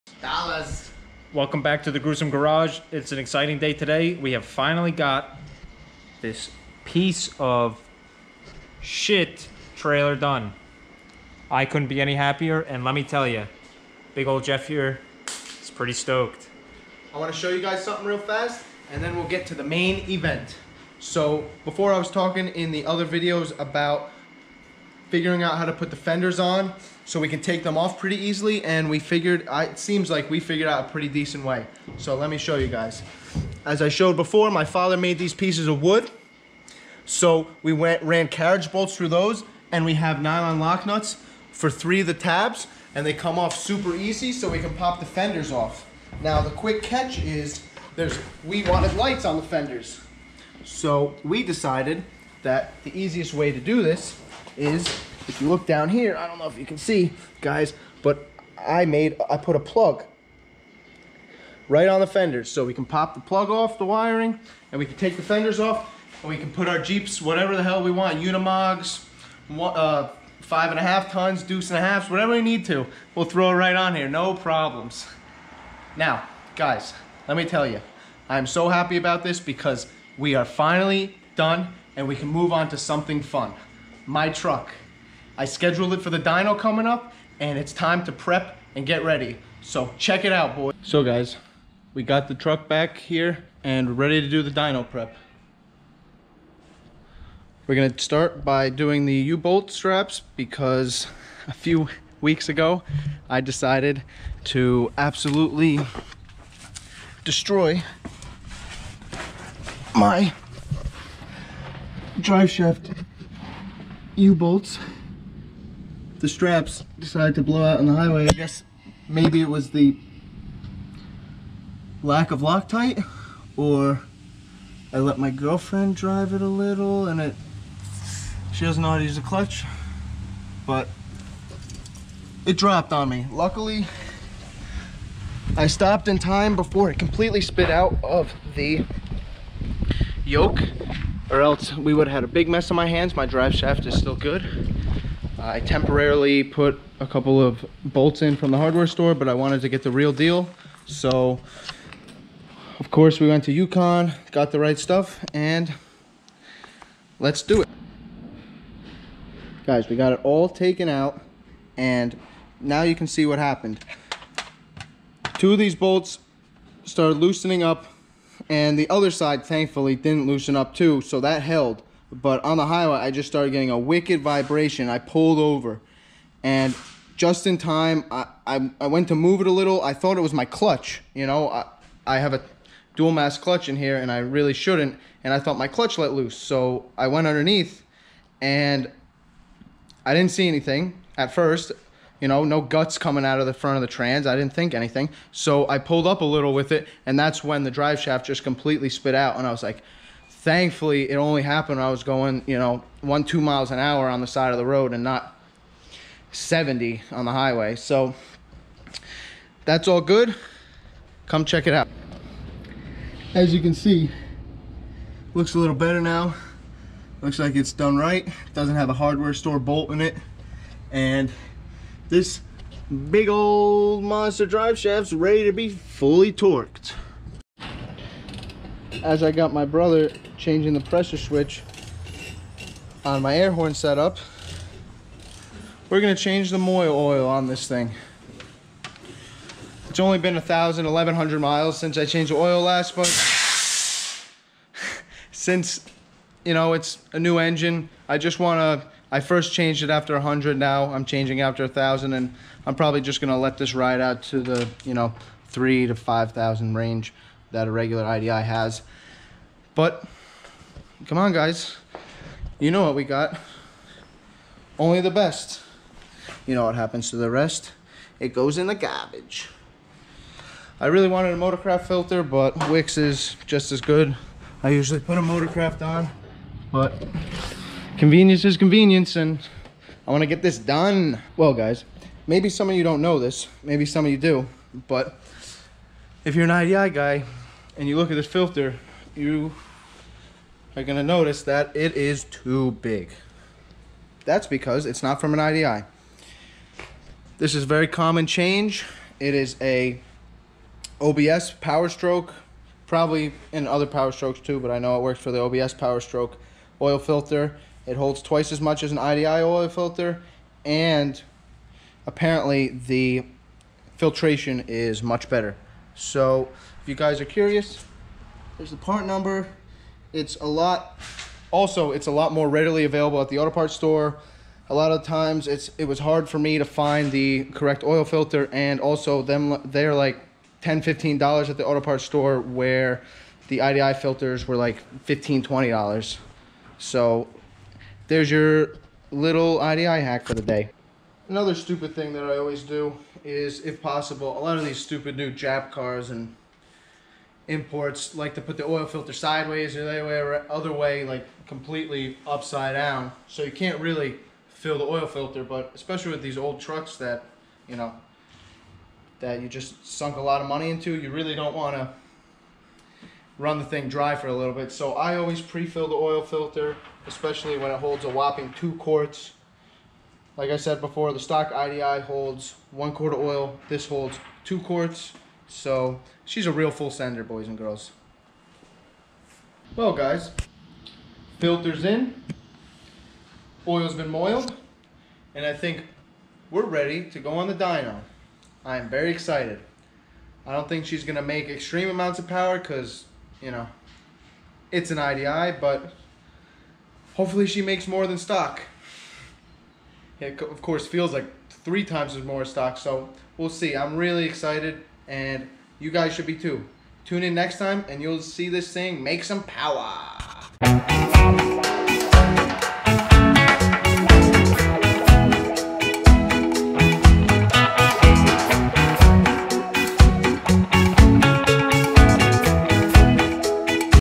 7.3 IDI Turbo Dyno Prep
Jeff's getting big Chungus his 1992 7.3 idi ready to try and make some power!

Engine specs: 92 7.3 idi na
Sleeved
Turbo pistons and rods
Total seal rings
Arp studs
R&D cam
R&D pump
R&D intake and exhaust manifolds
Comp cam valvesprings
Stock compression
Turbo: hx35/40hybrid
R&D downpipe